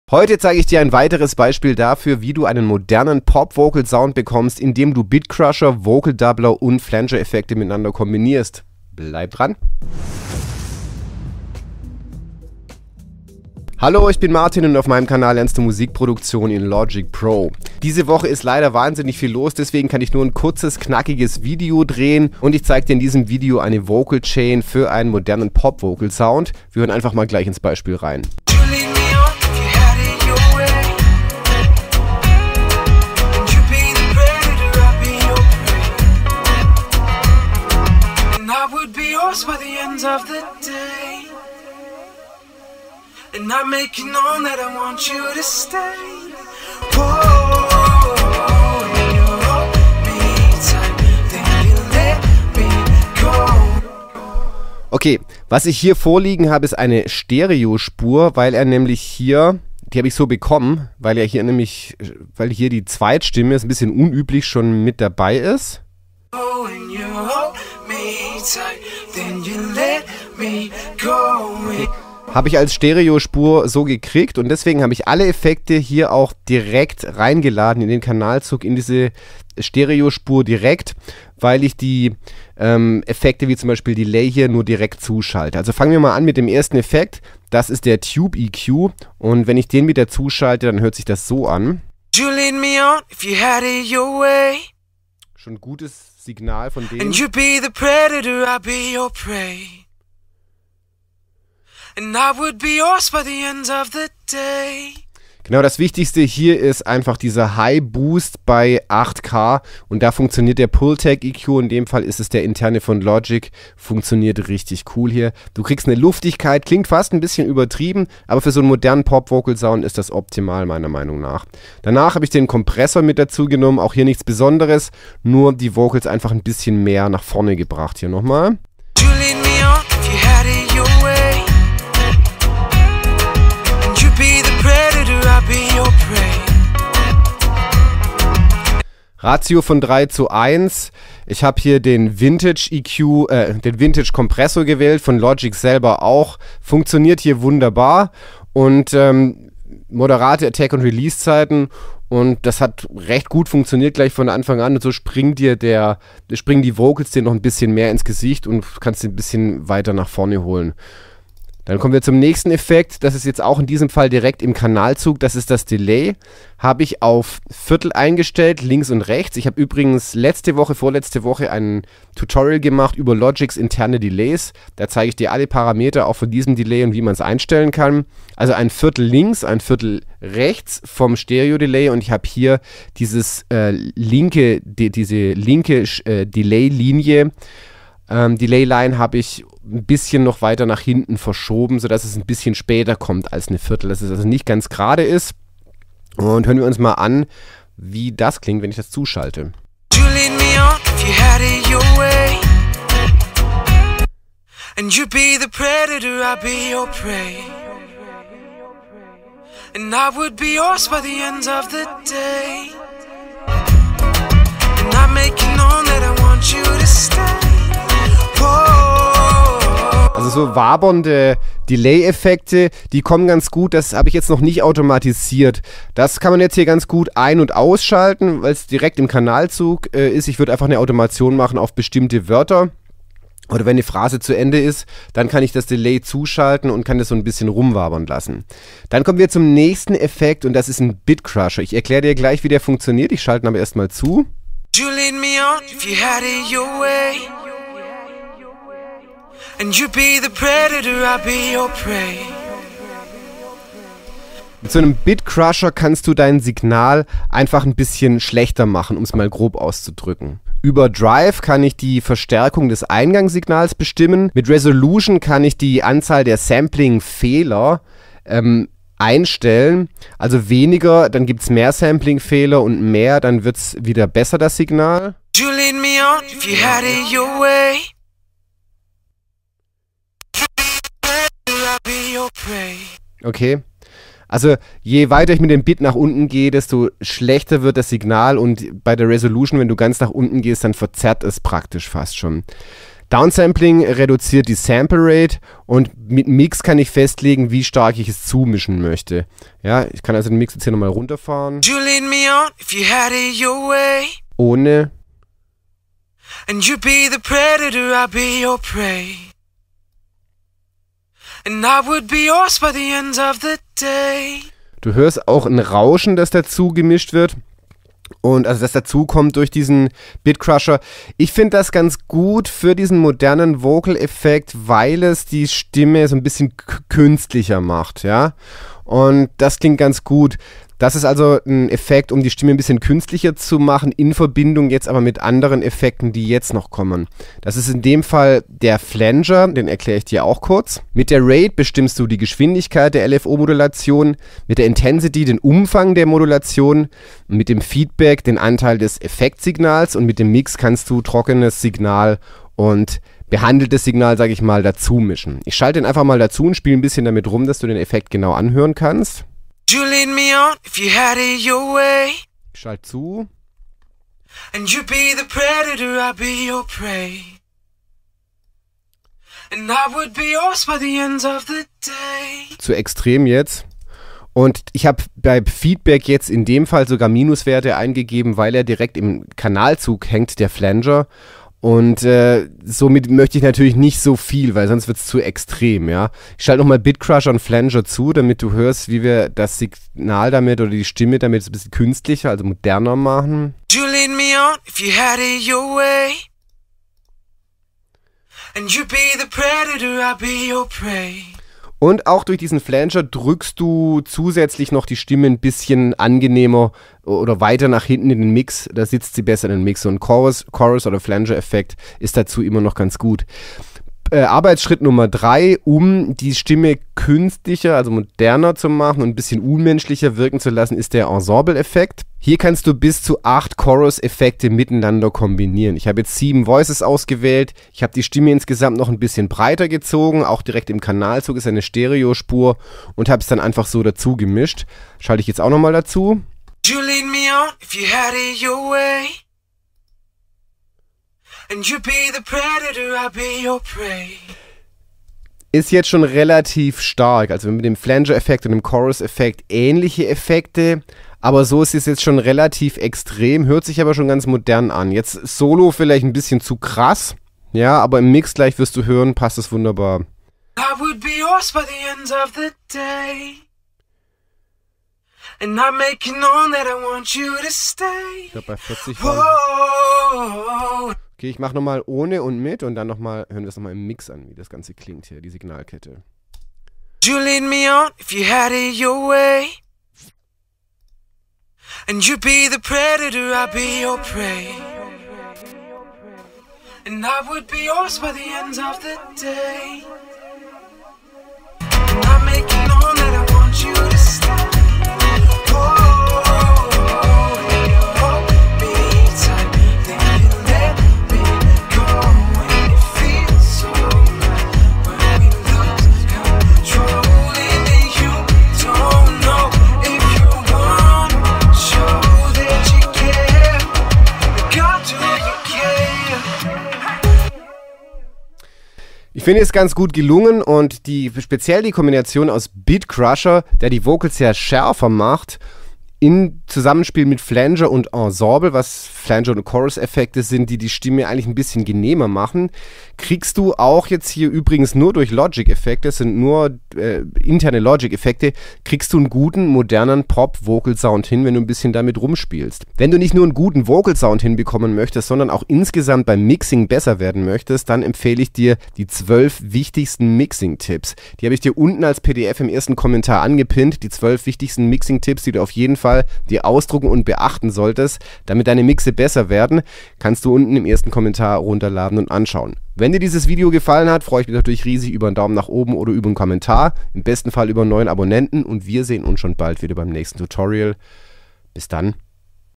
Vocals abmischen : Pro Pop Vocal Sound mit 6 Effekt Plugins || Song abmischen Tutorial
Möchtest du moderne Pop Vocals abmischen? Mit der Kombination aus Flanger, Chorus und Vocal Doubling Effekten erreichst du den großen & modernen Pop Vocalsound. Die komplette Praxisanelitung dafür gibt`s in diesem Songs abmischen Tutorial. Viel Spass!